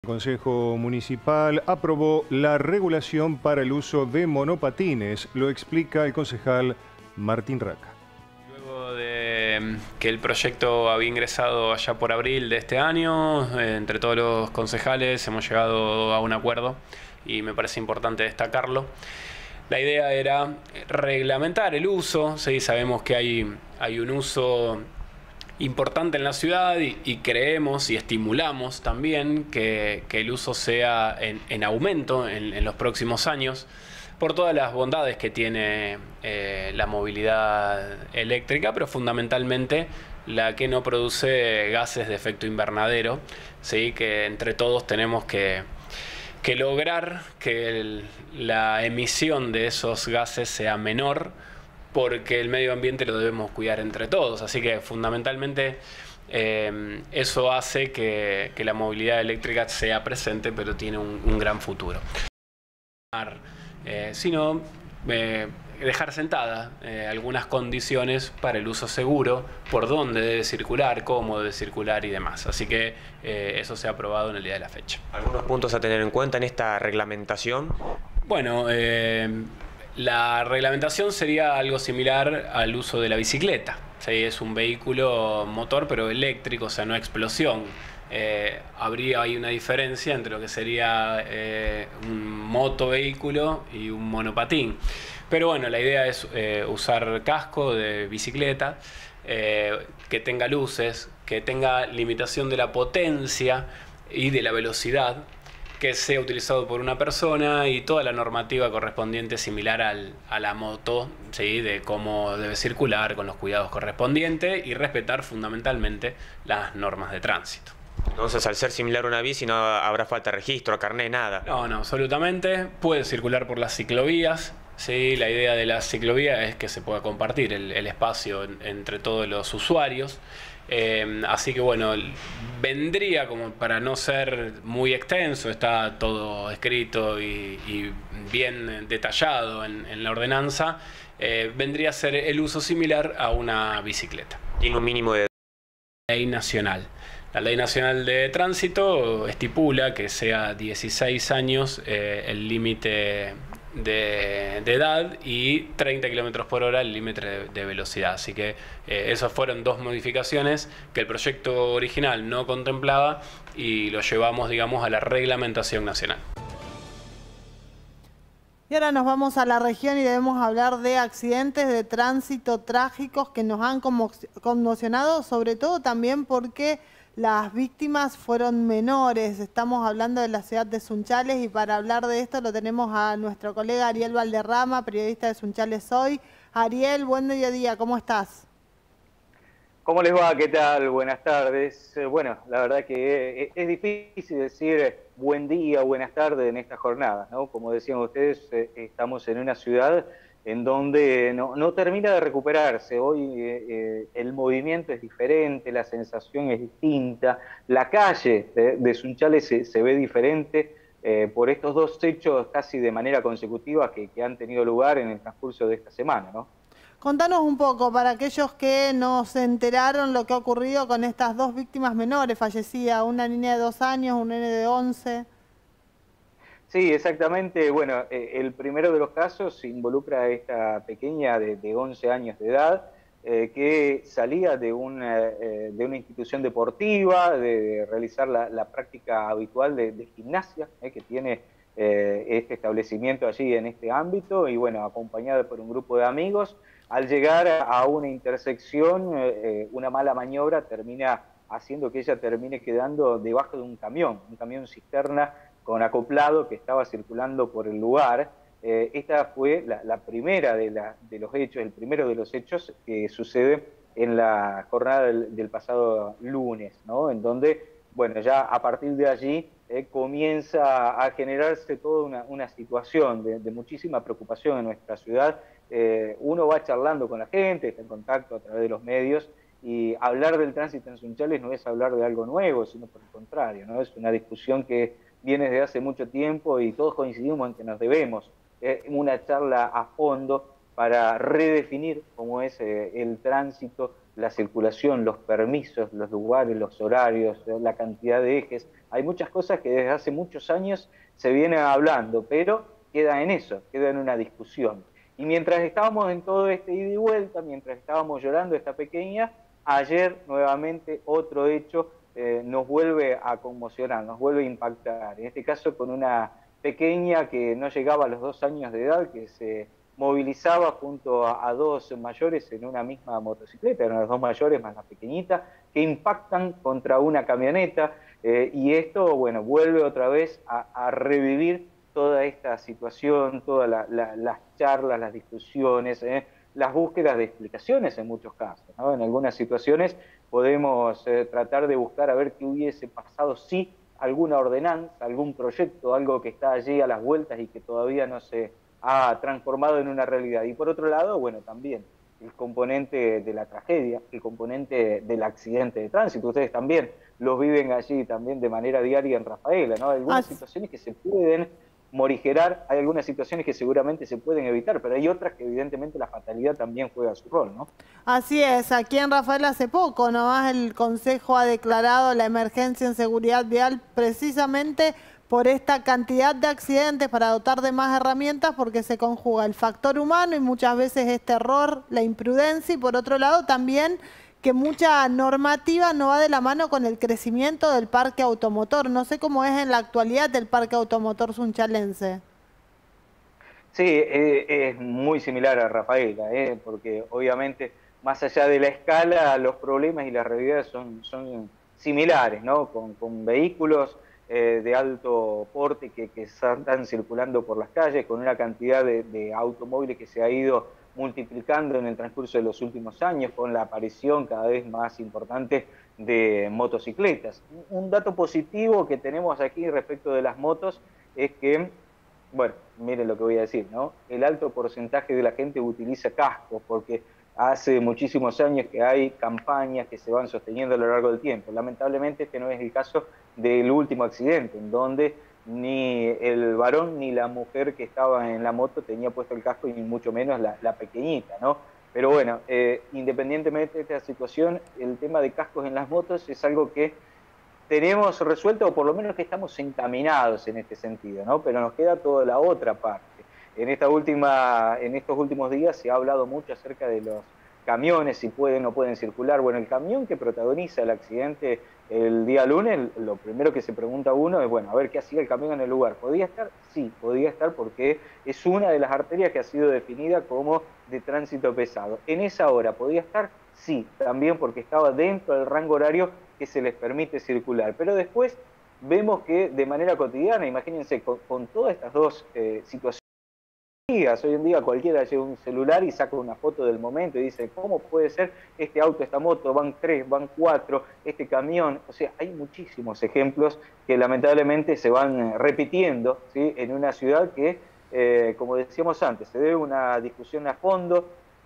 Next Noticias ACCIDENTES FATALES EN SUNCHALES
MARTÍN RACCA BRINDÓ PRECISIONES DE LA NUEVA NORMATIVA
EL CONCEJO APROBÓ LA REGULACIÓN PARA EL USO DE MONOPATINES.
ARIEL BALDERRAMA - PERIODISTA SUNCHALES HOY
TRÁGICOS ACCIDENTES SE PRODUJERON EN LA CIUDAD DE SUNCHALES